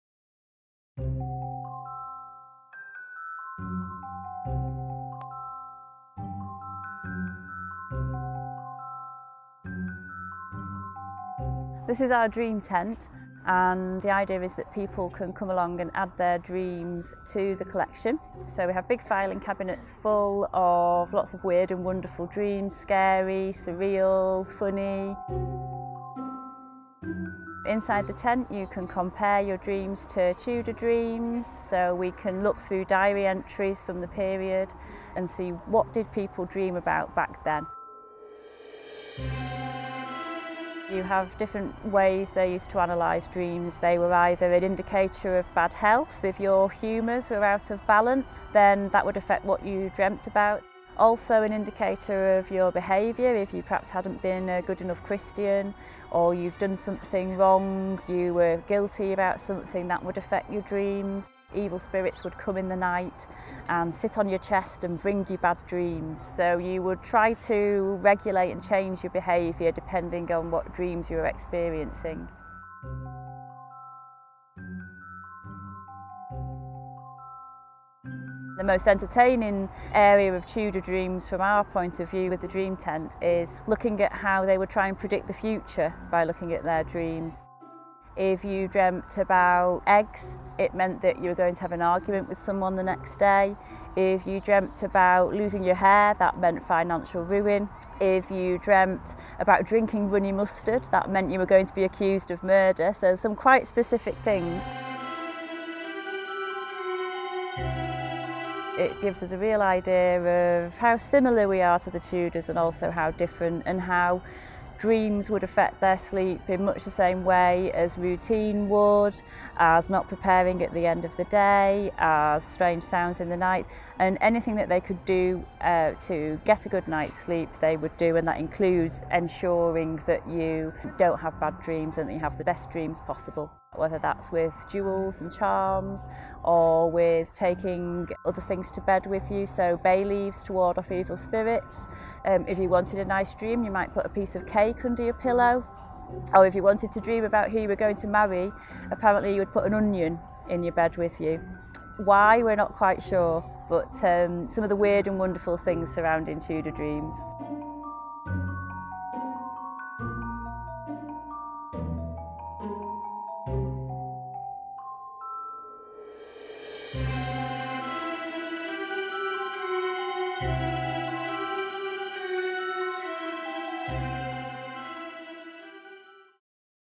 Little Moreton Hall The Dream Tent Film
Throughout 2017 the How We Used to Sleep project has featured the Dream Tent, an interactive space for visitors to write down or draw their dreams, add them to our dream collection, look at other people's dreams and learn about what dreams meant for the Tudors. Young and old have enjoyed thinking about the nature of dreams, whether people in the past dreamt in different ways to us and what signs and messages Tudors would watch out for in their dreams